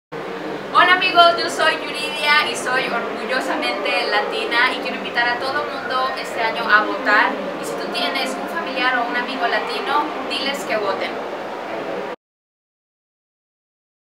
Yuridia DilesQueVoten
Este 8 de noviembre los latinos podemos cambiar el rumbo de las elecciones presidenciales en los Estados Unidos.
Si tienes familiares o amigos elegibles para votar:
Diles que se registren!!
Diles que voten!!